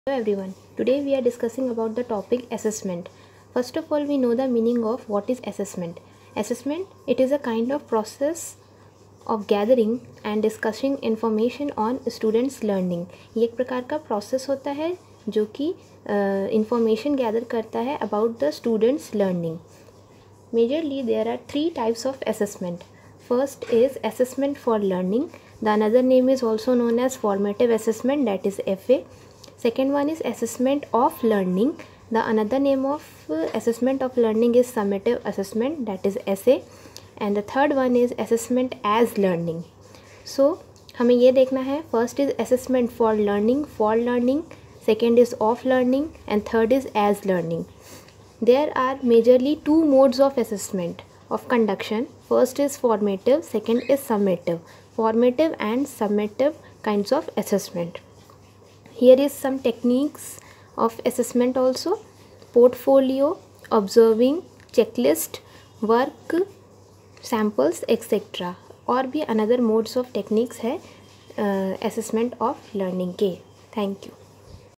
4 May 2020
Assessment of learning,types, modes and techniques.